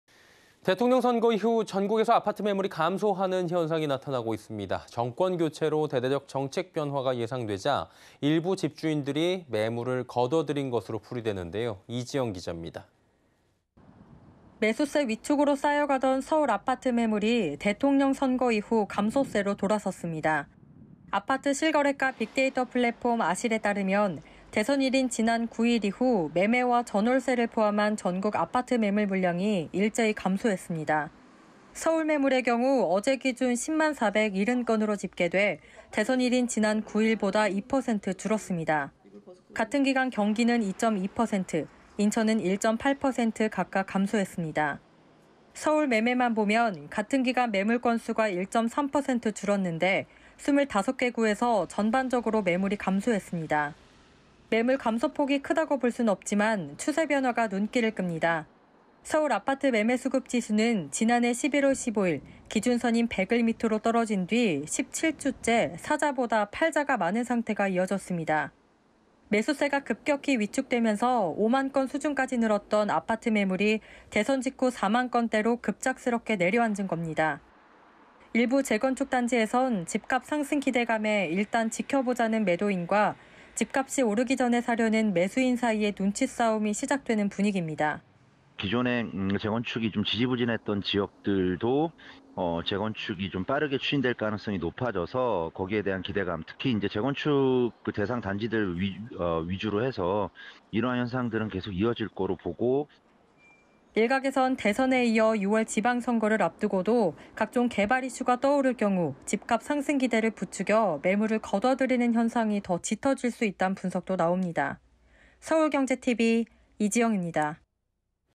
집주인들, 집값 상승 기대에 매물 회수
[앵커]
대통령 선거 이후 전국에서 아파트 매물이 감소하는 현상이 나타났습니다. 정권 교체로 대대적인 정책 변화가 예상되자, 일부 집주인들이 매물을 거둬들인 결과로 풀이됩니다. 이지영 기자입니다.

[기자]
매수세 위축으로 쌓여가던 서울 아파트 매물이 대통령 선거 이후 감소세로 돌아섰습니다.

아파트 실거래가 빅데이터 플랫폼 '아실'에 따르면 대선일인 지난 9일 이후 매매와 전월세를 포함한 전국 아파트 매물 물량이 일제히 감소했습니다.

서울 매물의 경우 어제(13일) 기준10만470건으로 집계돼, 대선일(10만2417건)인 지난 9일보다 2% 줄었습니다.

같은 기간 경기는 2.2%, 인천은 1.8% 각각 감소했습니다.

서울 매매만 보면 같은 기간 매물 건수가 1.3% 줄었는데, 25개구에서 전반적으로 매물이 감소했습니다.

매물 감소 폭이 크다고 볼 수는 없지만, 추세 변화가 눈길을 끕니다.

서울 아파트 매매수급지수는 지난해 11월15일 기준선인 100 밑으로 떨어진 뒤 17주째 '사자'보다 '팔자'가 많은 상태가 이어졌습니다.

매수세가 급격히 위축되면서 5만 건 수준까지 늘었던 아파트 매물이 대선 직후 4만 건대로 급작스럽게 내려앉은 겁니다.

일부 재건축 단지에선 집값 상승 기대감에 일단 지켜보자는 매도인과 집값이 오르기 전에 사려는 매수인 사이의 눈치싸움이 시작되는 분위기입니다.

[인터뷰] 최황수 건국대 부동산학과 교수
“기존의 재건축 지지부진했던 지역들도 재건축이 좀 빠르게 추진될 가능성이 높아져서 거기에 대한 기대감. 특히 재건축 대상 단지들 위주로 해서 이런 현상은 계속 이어질 거로 보고…”

일각에선 대선에 이어 6월 지방선거를 앞두고도 각종 개발 이슈가 떠오를 경우, 집값 상승 기대를 부추겨 매물을 거둬들이는 현상이 더 짙어질 수 있단 분석도 나옵니다. 서울경제TV 이지영입니다.

[SENTV]